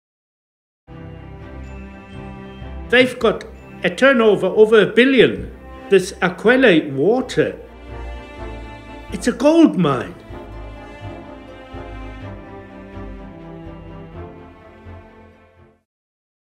PODCAST TEASER | EXODUS Chapter 4: Revival
A look at KwaSizabantu’s origins and its leader with a knack for enlisting God to heal the sick.